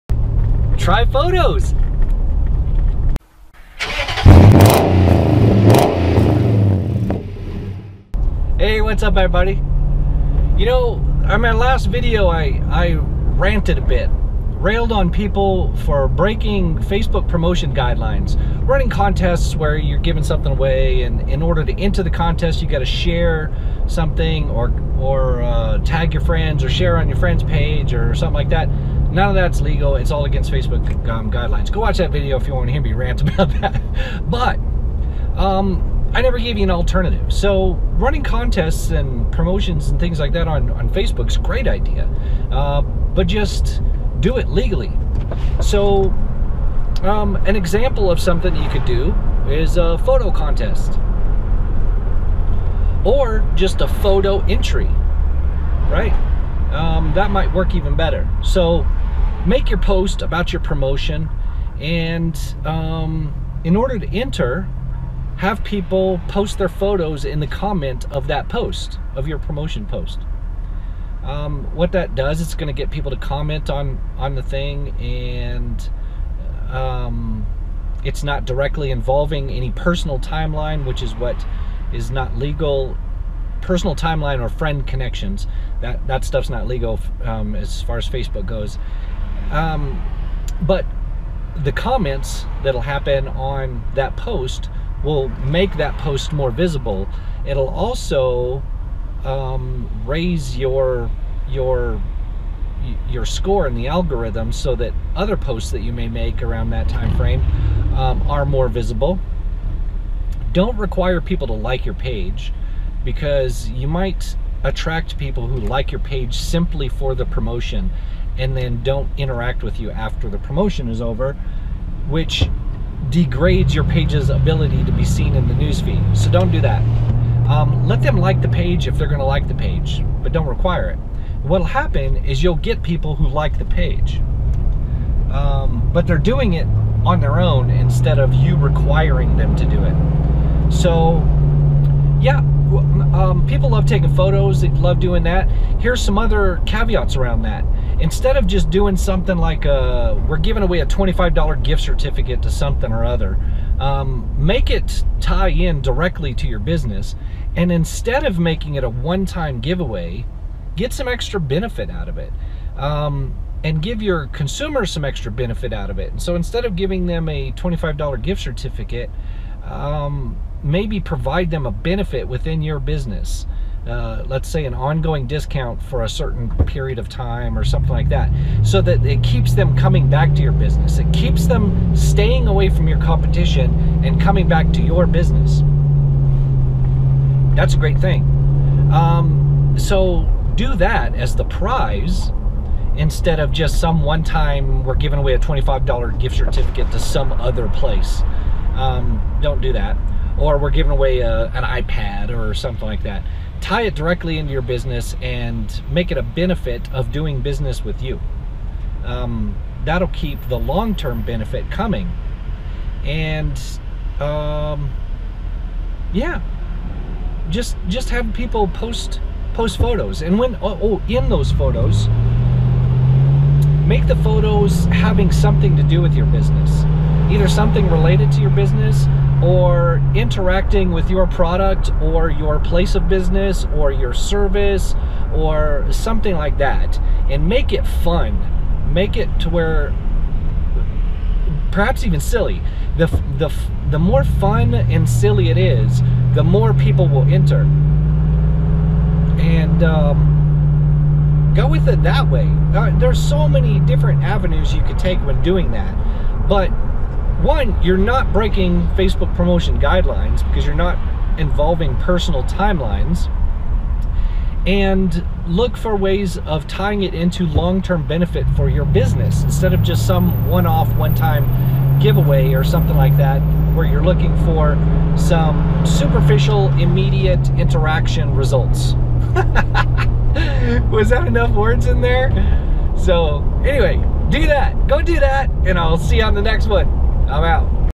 Ken Drives: Running a Facebook Contest
If you want to run a Facebook contest, ensure you take these things into consideration so you don't break the rules and/or harm you page in the long run.